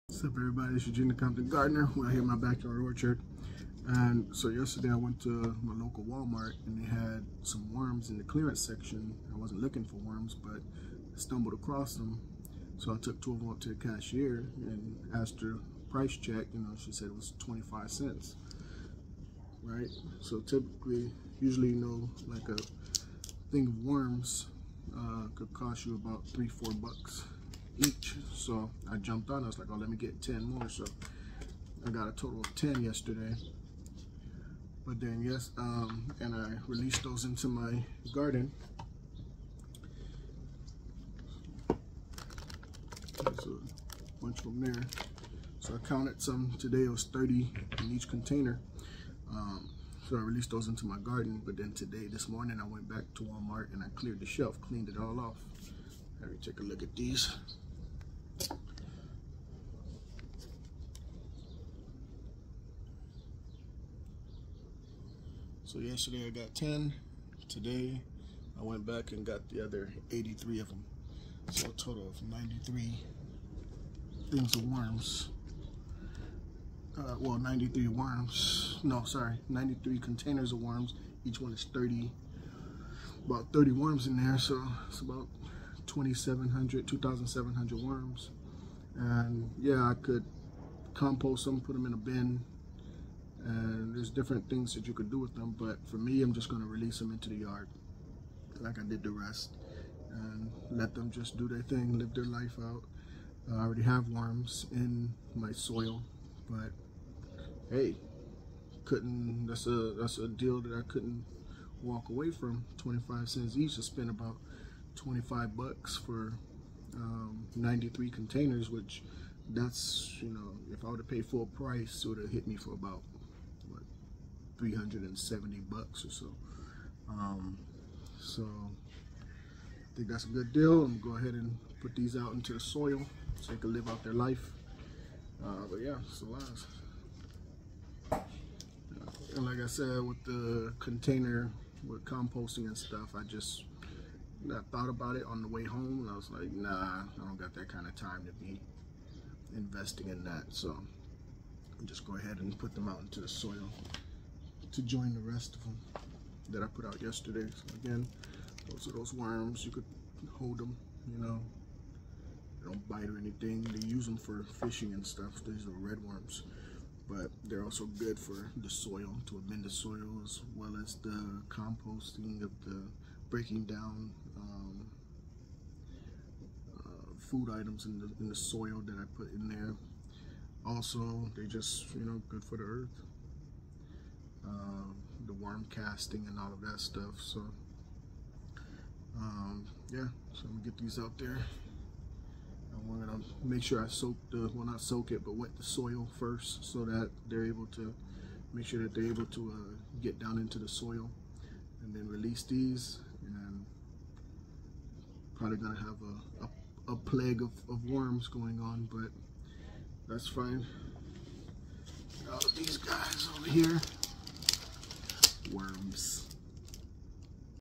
Releasing 2,700 red worms
releasing 2,700 worms into my garden.